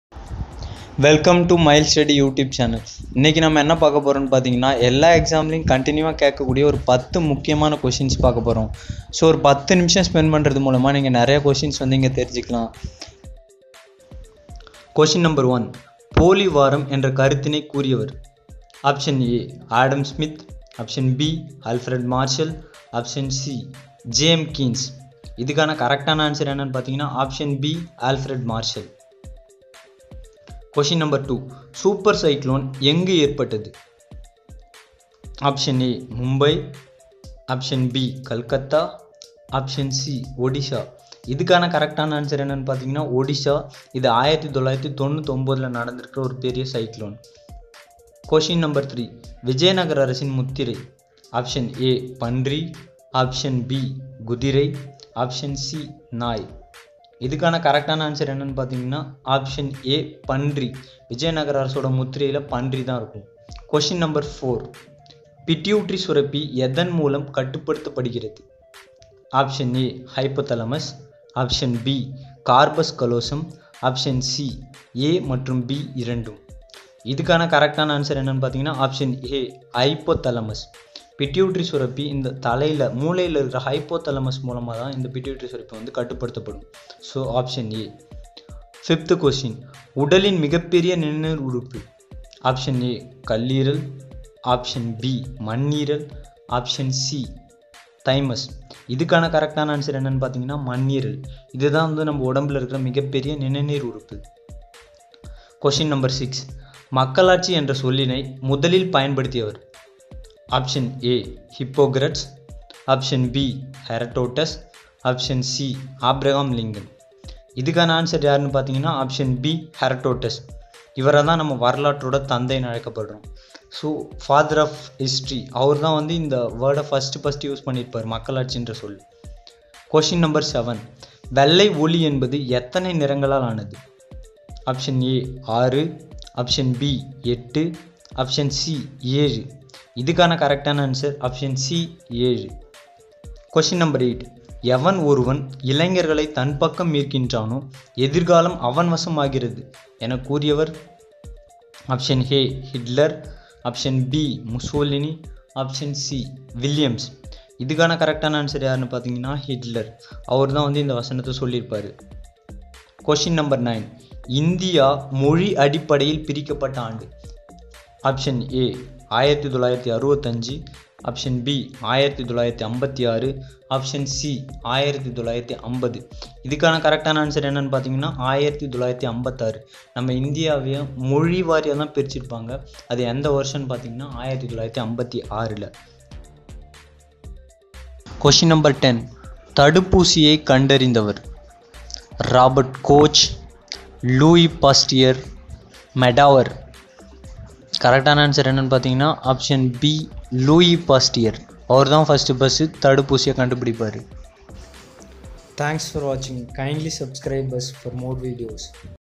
Important GK questions and answers Day - 1
In this video I explained 10 important static  Gk questions for all govt exams .. kindly follow us for more videos .. comment the number of questions you answered before i tick the answer.. thanks for watching..! Have a great day ...!